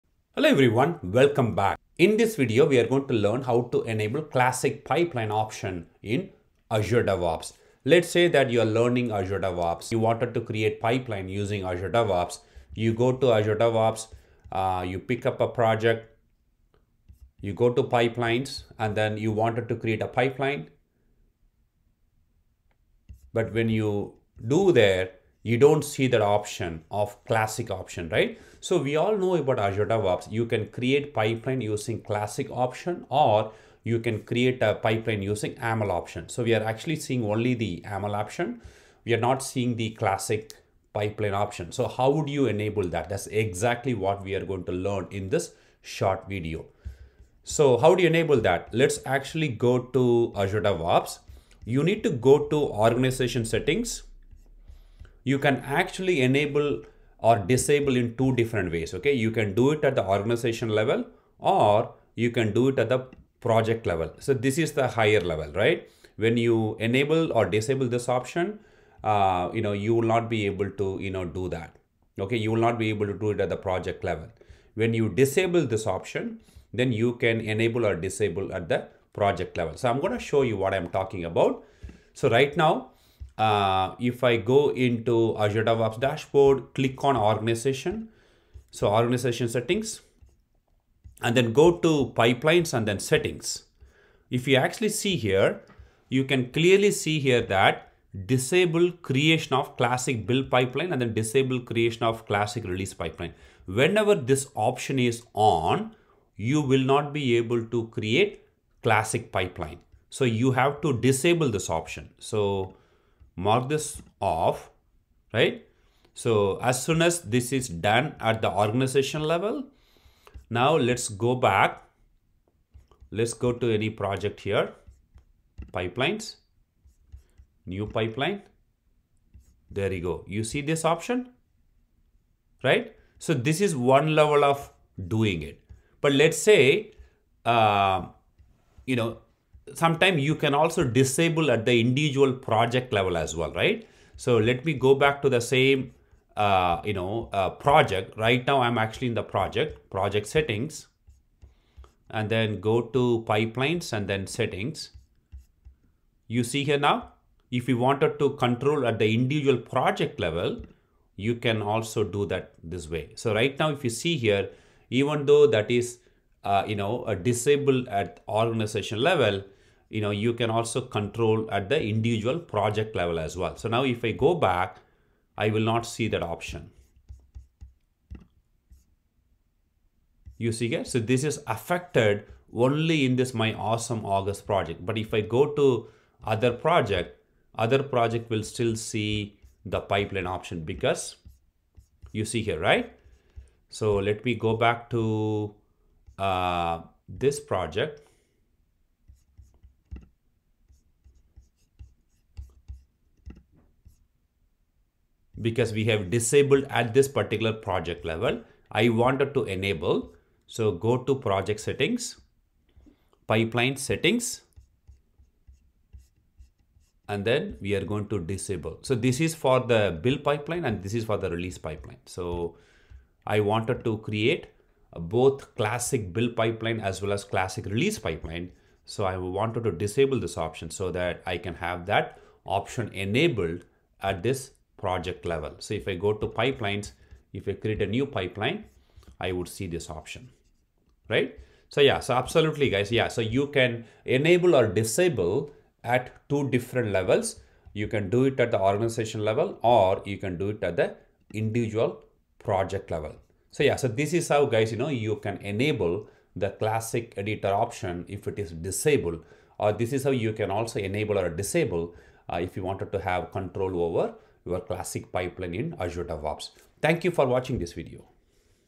Enable Classic Pipeline in Azure DevOps | How to Enable classic editor create pipeline without YAML
How to enable the classic build and release pipelines?

You can enable/disable it two ways
Organizational level
Project Level
In Azure DevOps,  Go to Project Settings, Under Pipelines - Settings - General. Make sure 'Disable creation of classic build pipelines' and 'Disable creation of classic release pipelines' is turned off, to have classic editor shown after creating a project.

If those options are disabled, go to organizational level and do the same in 'Organization Settings'.